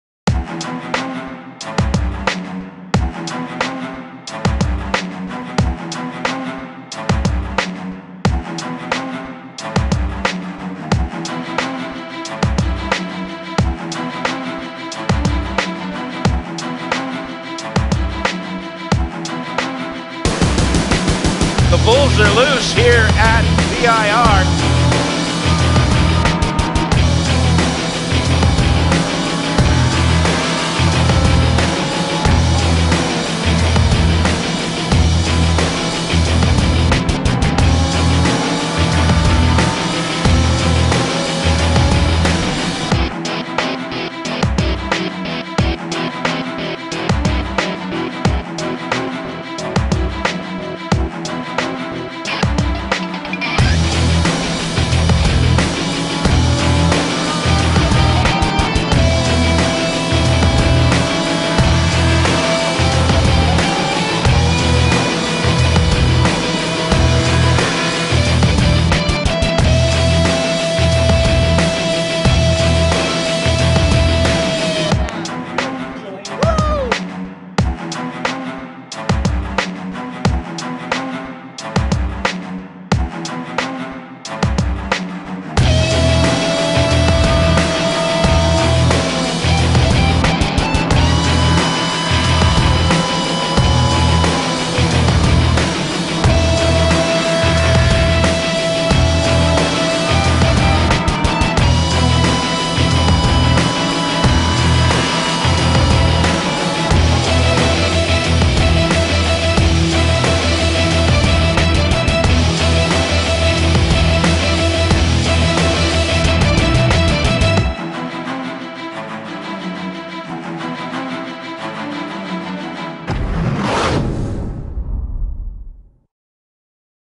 Lamborghini Super Trofeo North America - Virginia Int'l Highlights 2015 - Unravel Travel TV
Richard Antinucci and Edoardo Piscopo ran away from the field to capture their second Lamborghini Blancpain Super Trofeo North America victory of the weekend, winning Round 6 on Sunday 23rd August 2015 at VIRginia International Raceway. Antinucci and Piscopo took the checkered flag under caution in the No. 50 Lamborghini Huracán LP 620-2 Super Trofeo representing Lamborghini Beverly Hills and fielded by O'Gara Motorsport.

"Another great race, a great win," Antinucci said. "(Piscopo) brings a little bit of luck and a lot of ability. He teaches me a lot, and we complement each other really well. Ultimately he's capable of being faster than everyone else out there and then hands me a car with some of the freshest tires in the field."

Local team Change Racing rounded out the top-three finishers on the 17-turn, 3.27-mile circuit with Justin Marks finishing second in the No. 99 Lamborghini and Corey Lewis finishing third in the No. 29 Lamborghini. Lewis was also the top Pro-Am finisher.